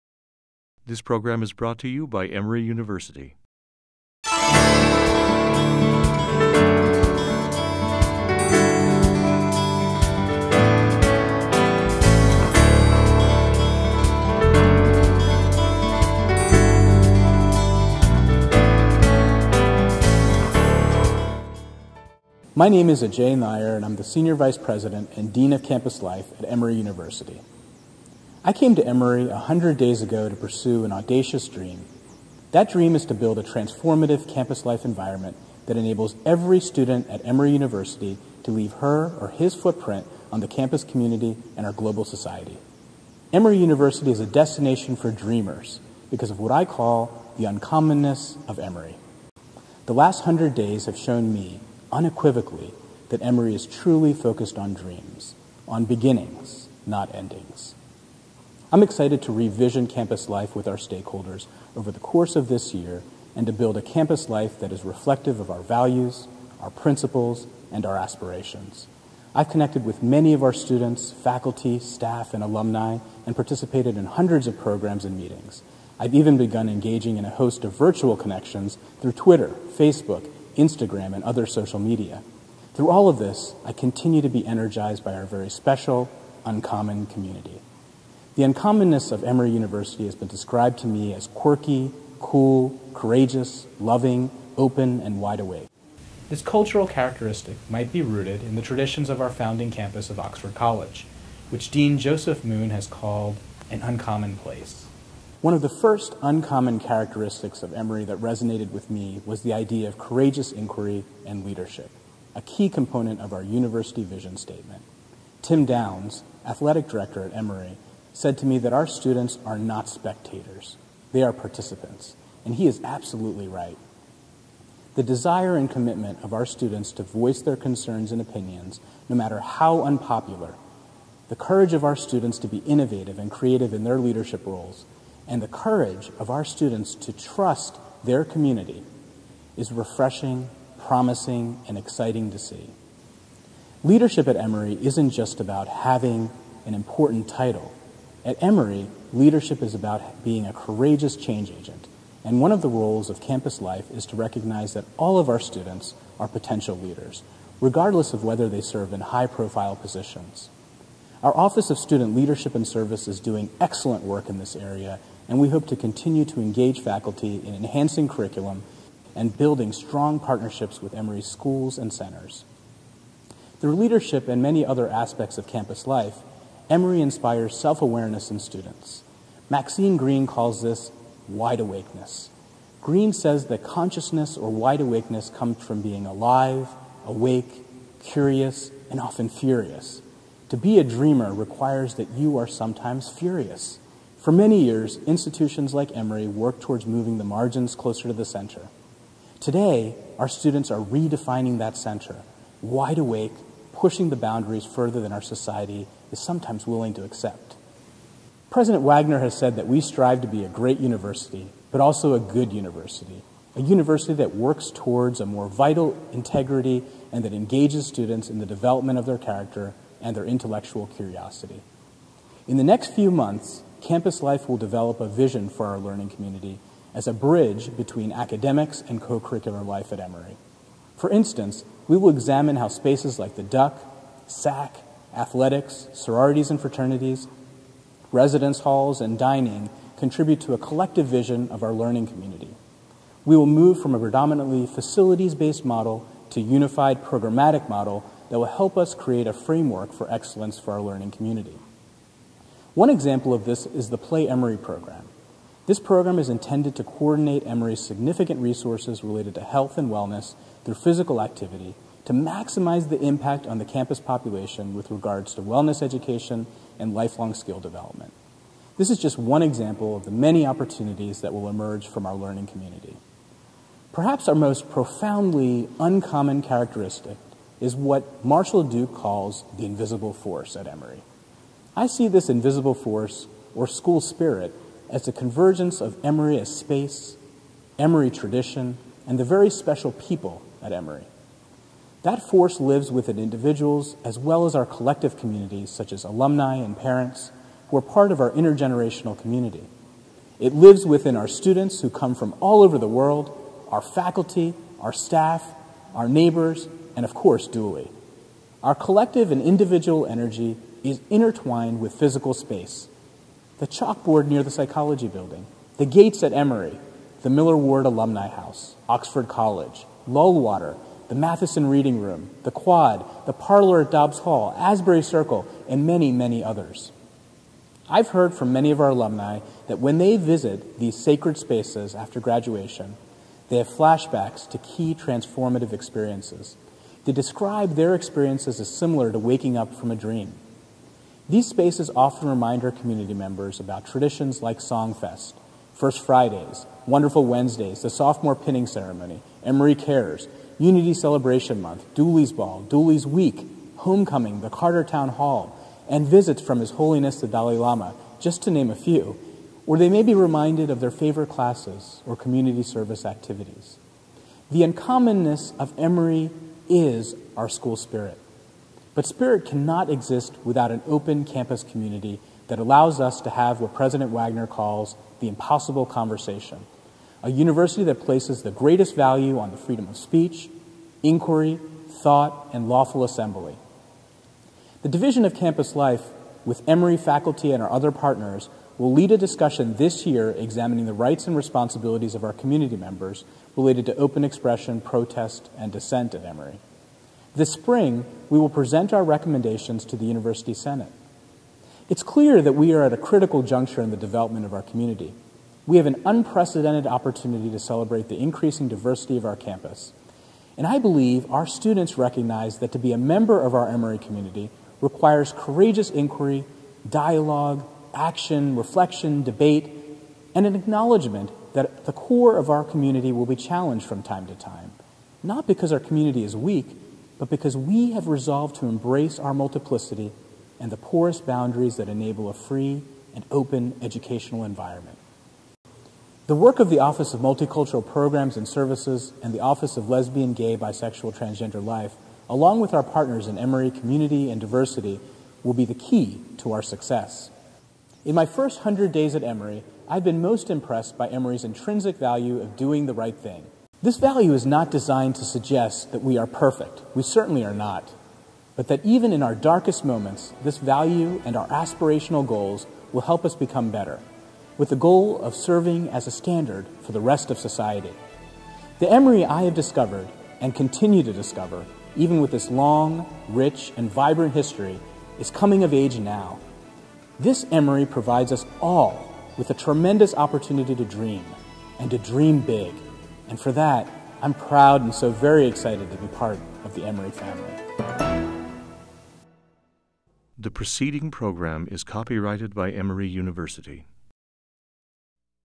The Uncommonness of Emory
Senior Vice President and Dean of Campus Life Ajay Nair reflects on his first 100 Days at Emory University.